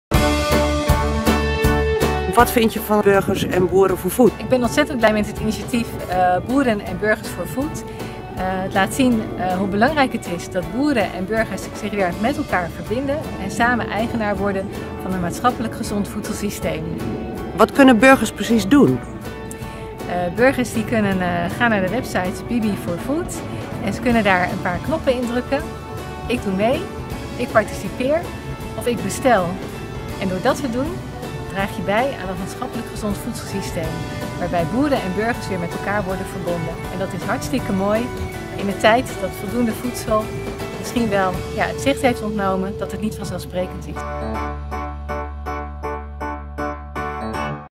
BB4Food Snack#2 Even voorstellen! Carla Dik
De komende weken stellen we een aantal mensen aan u voor die zeer actief zijn binnen het project Burgers & Boeren 4 Food of het project een warm hart toedragen. Deze week Carla Dik, Tweede Kamerlid van de ChristenUnie. Zij was gelijk enthousiast over ons project, zeker gezien het feit dat in het regeerakkoord ook de verbinding burgers en boeren voor een gezonder voedselsysteem hoog in het vaandel staat.
Carla: “Het project BB4Food laat zien hoe belangrijk het is dat boeren en burgers zich weer met elkaar verbinden en samen eigenaar worden van een maatschappelijk gezond voedselsysteem. En dat is hartstikke mooi in een tijd dat voldoende voedsel misschien wel het zicht heeft ontnomen, dat het dagelijks beschikken over voedsel niet vanzelfsprekend is”.

Carla zet haar grote netwerk in om het project te laten slagen.
U kunt dit ook doen door dit bericht te delen en ook daadwerkelijk te doneren op